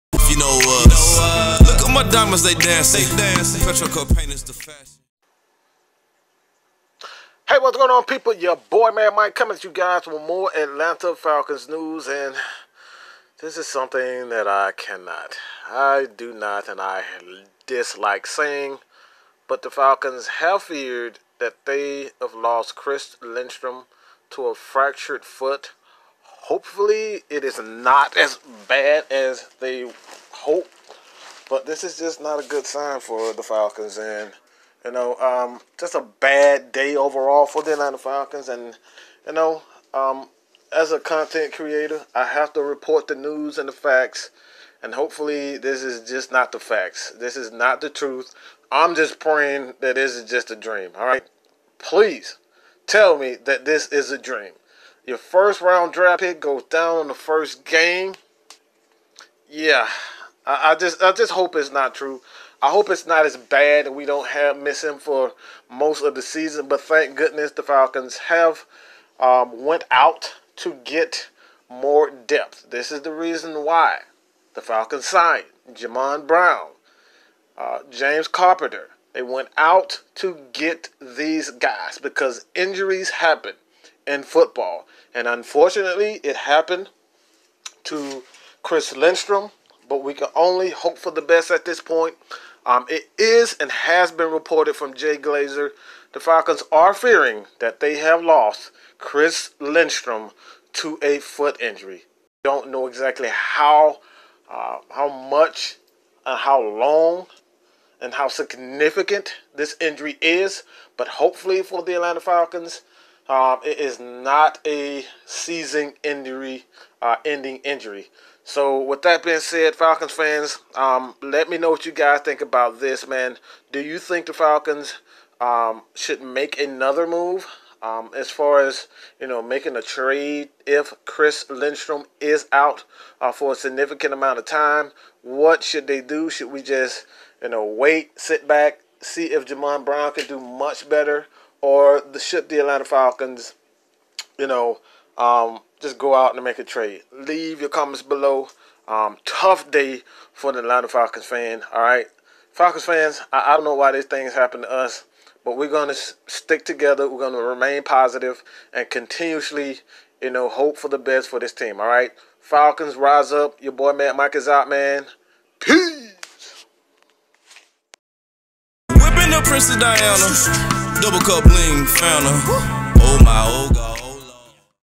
Atlanta Falcons News | Falcons Fear Chris Lindstrom Foot Injury | Mad Mike Sports
Atlanta Falcons News is the topic at hand. Chris Lindstrom left the first game with an injury.